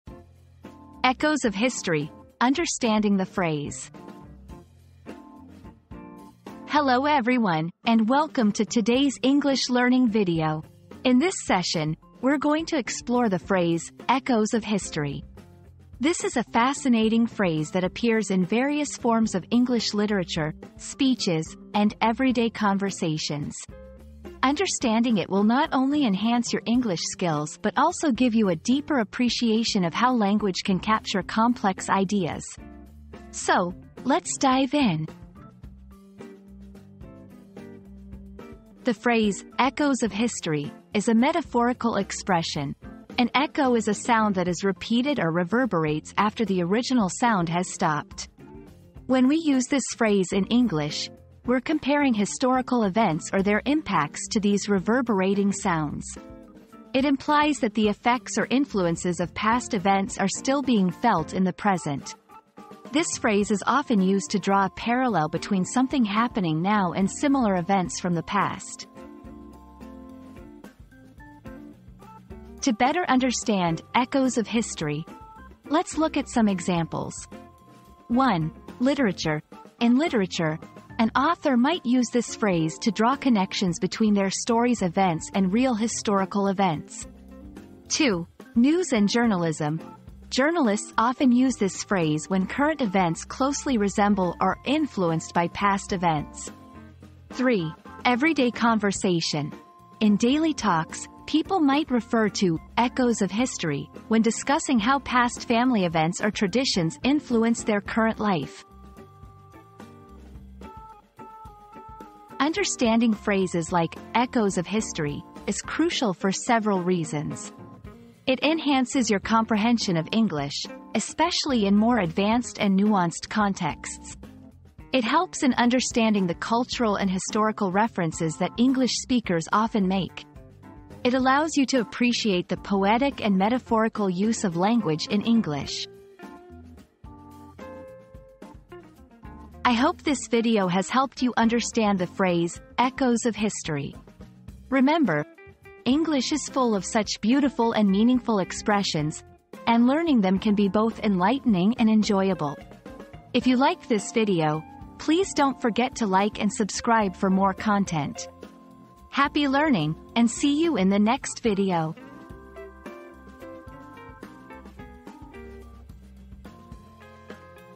Echoes of History: Understanding the Phrase
00:00 • Introduction - Echoes of History: Understanding the Phrase
00:38 • What Does "Echoes of History" Mean?
01:12 • Examples in Context
01:51 • Why is it Important?